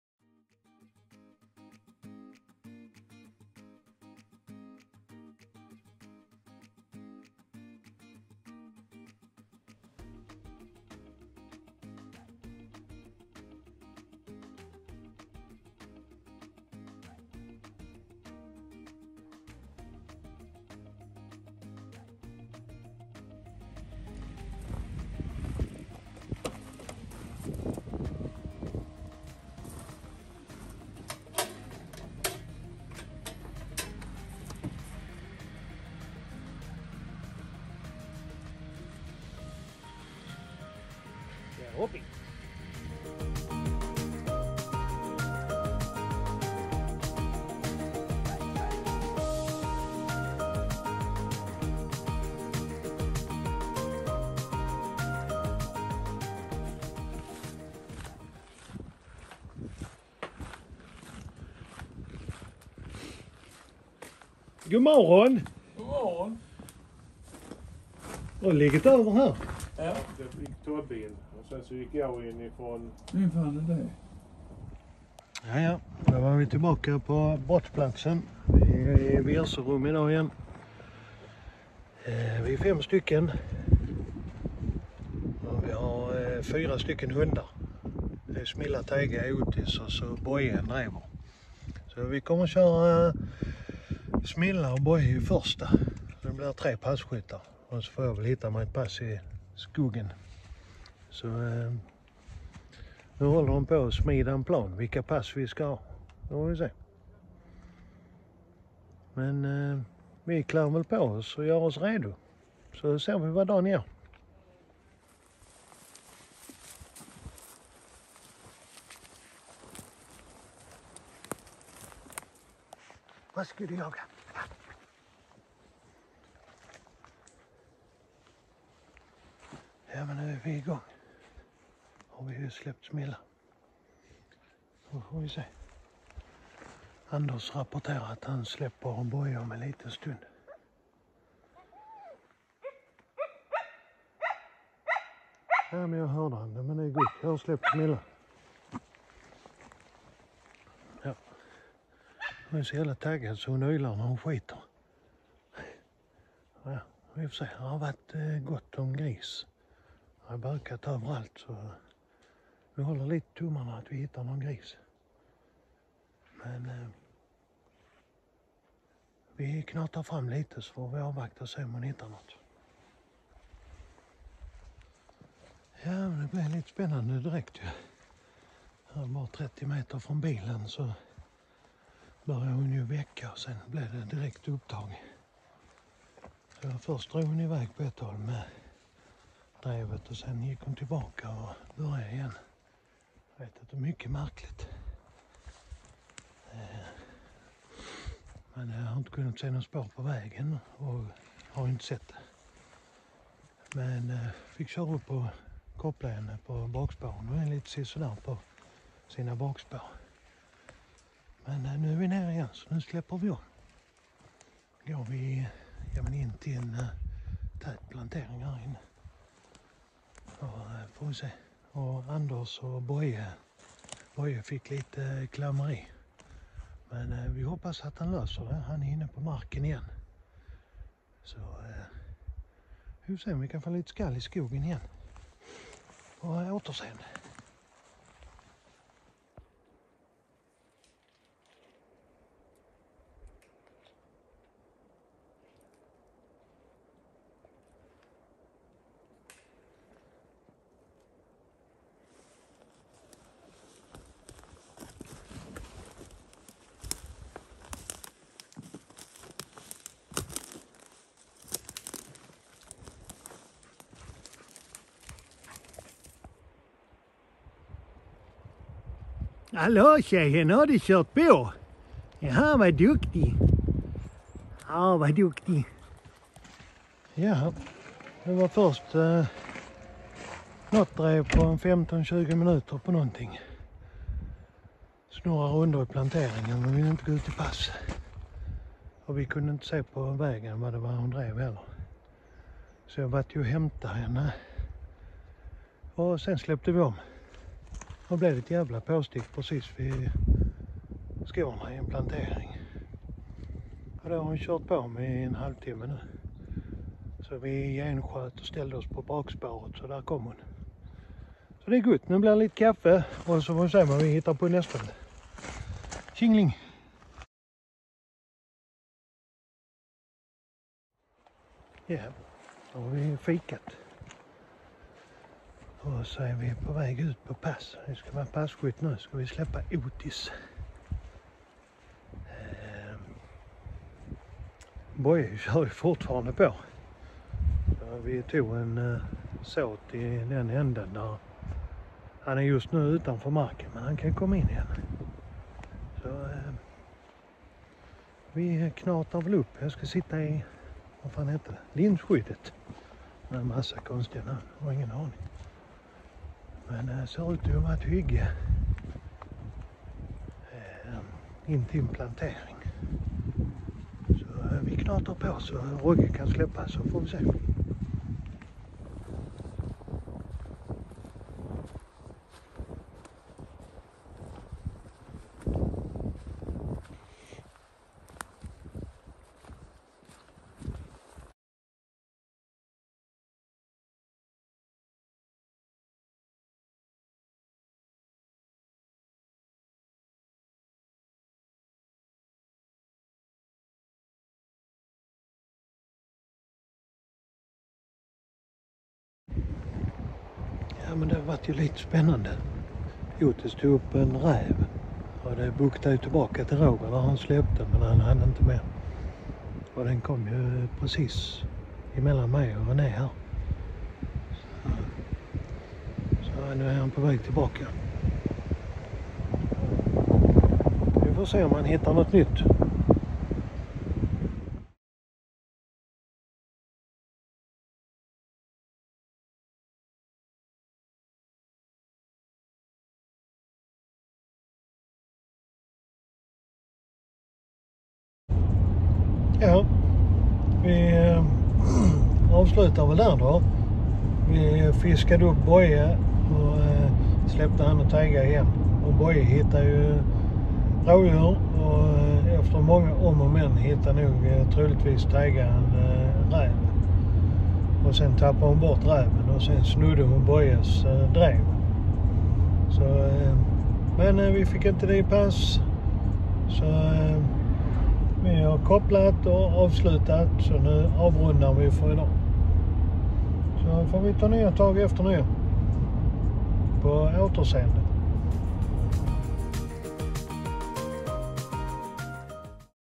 Lite jakt innan nyår.
Jakt på hemmamarken innan nyår.